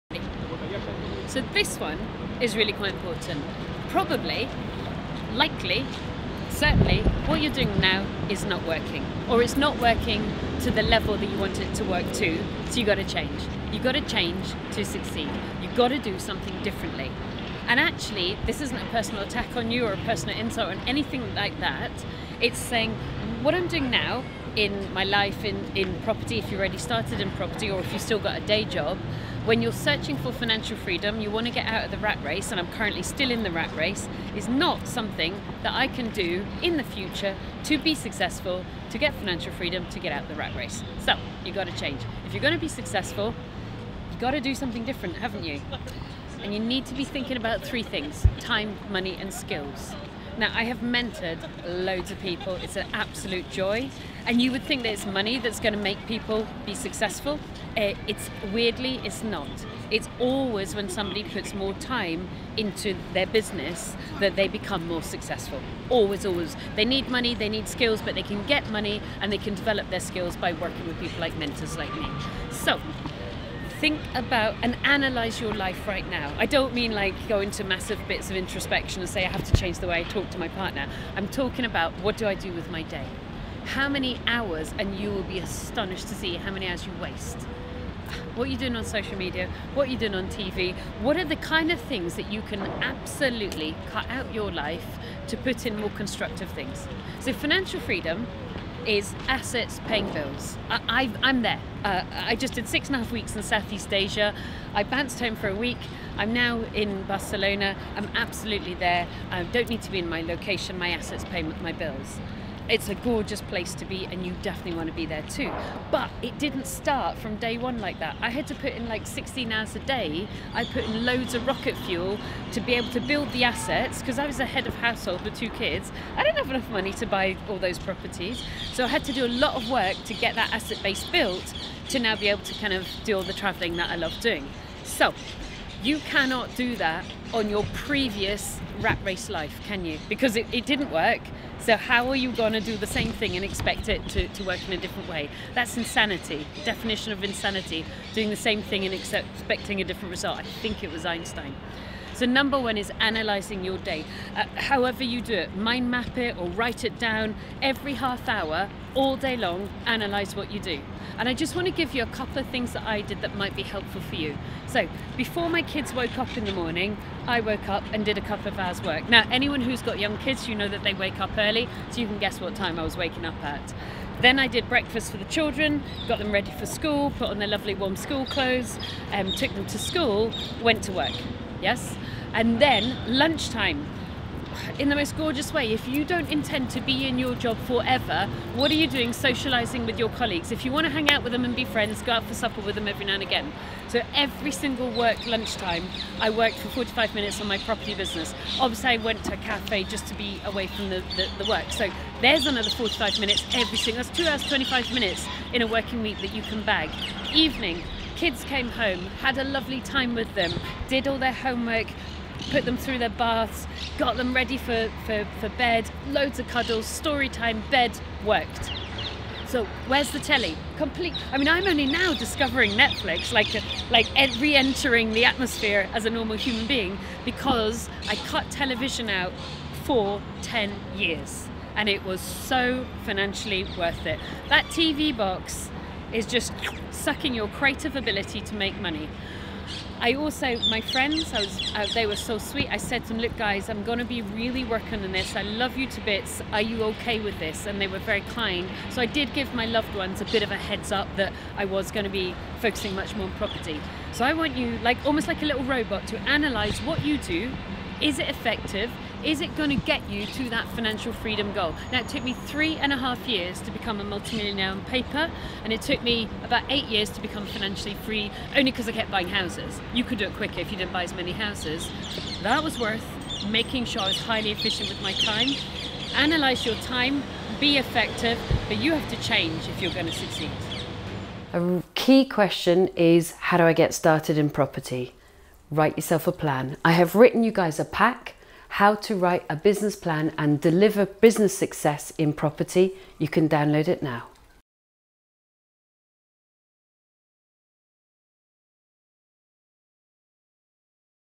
You Have To Be Different To Succeed
If you are going to be successful, you have got to be different. If you keep doing the same things and expecting a different result, you are doing it all wrong. It is important that you make changes to help grow your property business.

Not sure what changes to make? The first step is to analyse what you are currently doing. How is it helping your business and is it necessary?

Analyse your day. Break it down and see where improvements can be made.

I wrote my business planning pack to help you get your business plan down on paper.